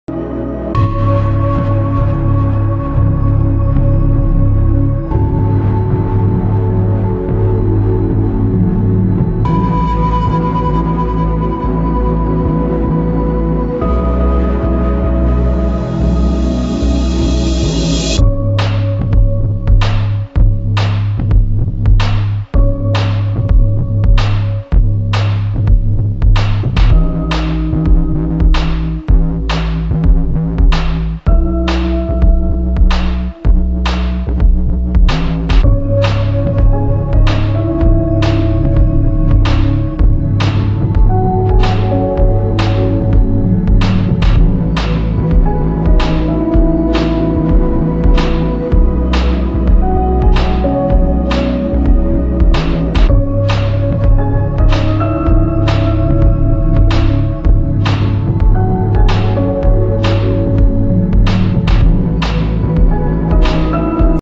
Mi 11 Ultra || Super Moon Feature || 60x Zoom || Best Camera Feature
The Lines were clearly visible and it was indeed beautiful, watching the moon from so close...